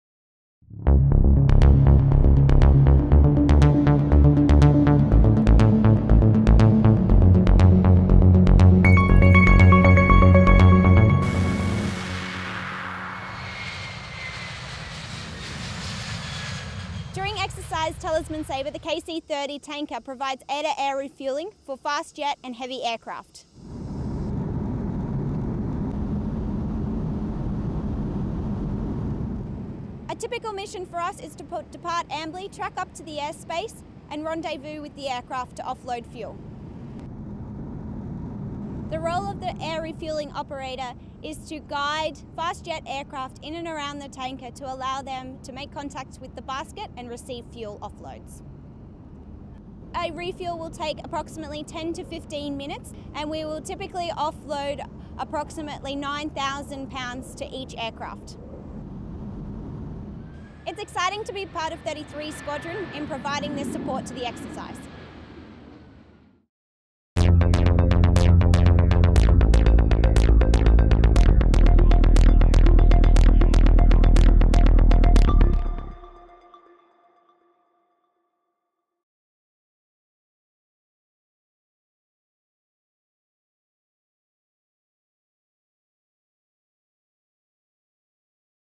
Talisman Sabre 2017 – how KC 30A palys its part
RAAF's KC-30A Multi Role Tanker Transport aircraft played its part during Exercise Talisman Sabre 17 keeping Australian and American fighter jets in the air for longer.

RAAF Base Amberley was the main operating base for RAAF and United States aircraft activities during Talisman Saber 2017 (TS17).

EA-18G Growlers from RAAF's No 6 Squadron and the United States Navy's VAQ-132 Scorpion Squadron were required to spend long hours on station over central Queensland during key phases of the exercise.

Exercise Talisman Sabre 2017 (TS17) is a biennial combined Australian and United States training activity, designed to train respective military forces in planning and conducting combined task-force operations to improve the combat readiness and interoperability between the respective forces.

This exercise is a major undertaking that reflects the closeness of the two alliance and the strength of the ongoing military-to-military relationship.

The exercise involves more than 30,000 soldier, sailors, airmen and marines.

Now in its seventh iteration, the Talisman Sabre series of exercises is the principal Australian and US military training activity focused on the planning and conduct of mid-intensity high-end warfighting in the air, land and maritime domains.

TS17 incorporates force preparation activities, special-forces activities, amphibious landings, parachuting, land-force manoeuvre, urban operations, air operations, maritime operations and a number of live-fire components.

Sincerely,

Brian Hartigan
Managing Editor

Minnamurra
NSW 2533
AUSTRALIA